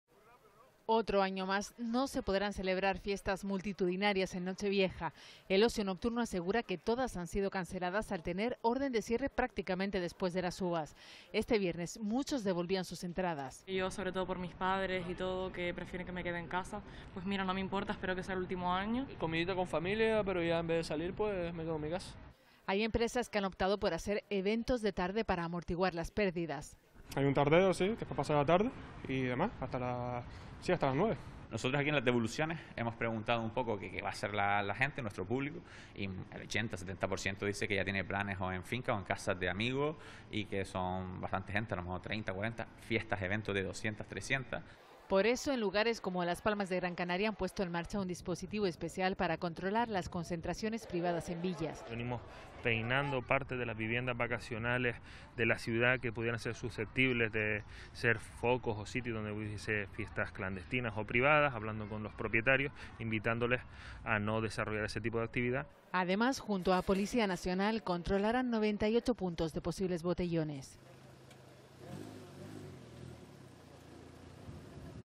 La pandemia marca las celebraciones de fin de año
Un año más la pandemia vuelve a marcar las celebraciones de fin de año. Muchas fiestas se han suspendido por las limitaciones de horario y de los aforos en interiores.

El ocio nocturno asegura que todas las fiestas han sido canceladas al tener orden de cierre prácticamente después de las uvas. Este viernes muchos devolvían sus entradas.

 Informa: Beatriz G. Cabrera / Elena Pulido